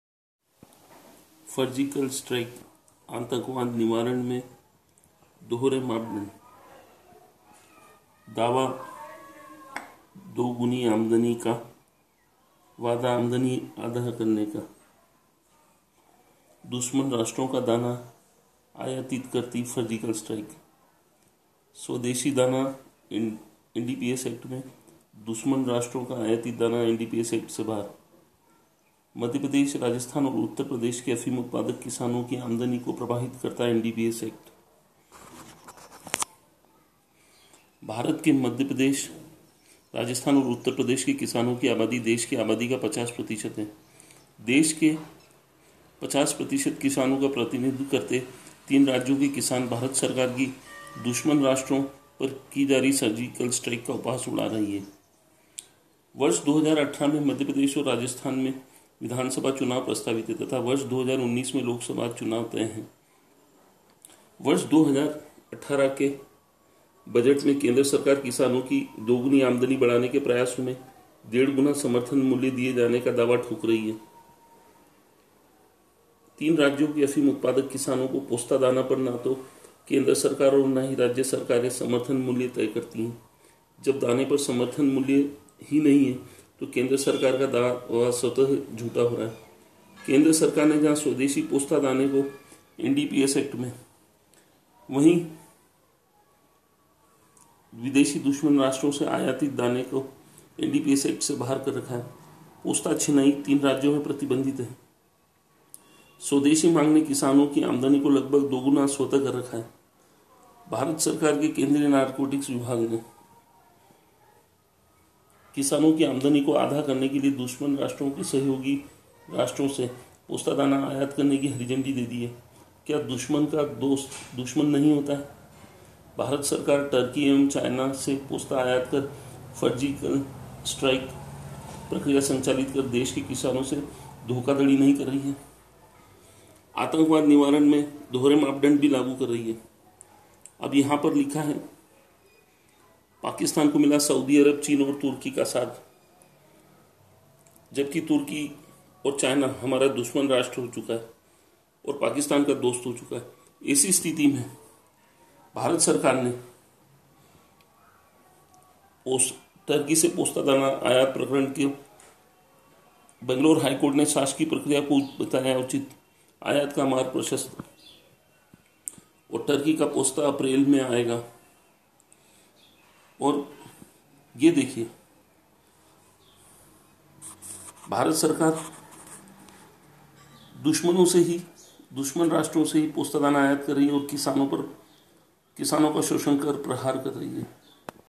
DAWA DOGUNI KA,WADA AAMDANI AADHI KARNE KA
DUSHMAN RASHTRO CHINA AUR TURKI SE POSTA DANA AAYAT KARNE KI FARJIKAL STRIKE